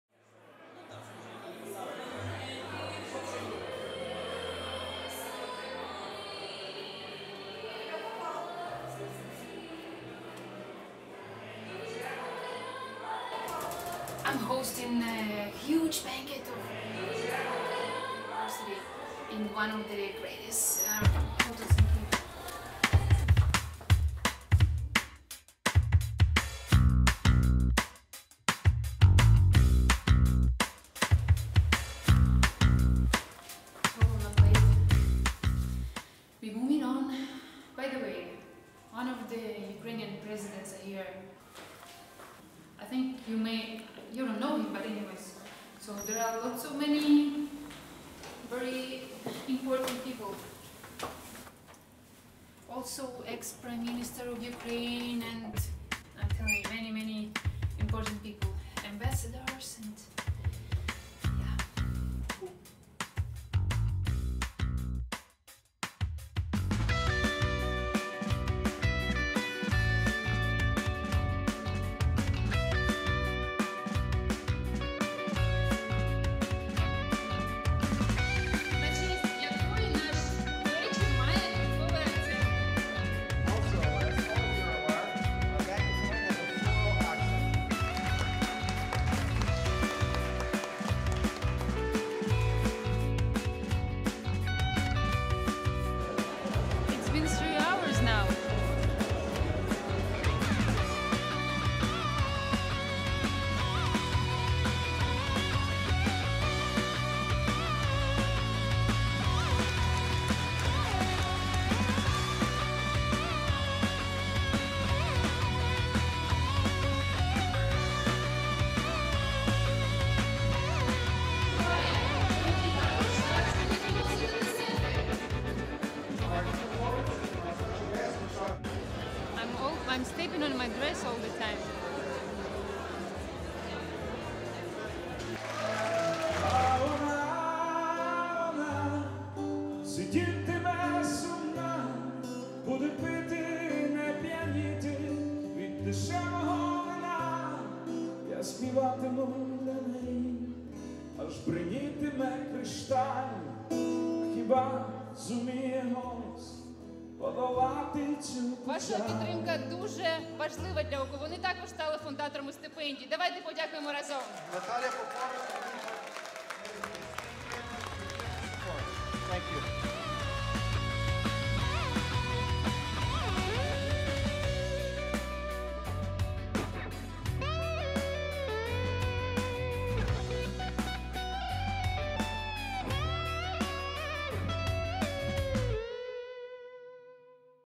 All Ukrainian celebrities and politicians in one place, can you believe?!
Masha Butenko was hosting a banquet full of ukrainian celebrities and poiliticians. See how she managed it!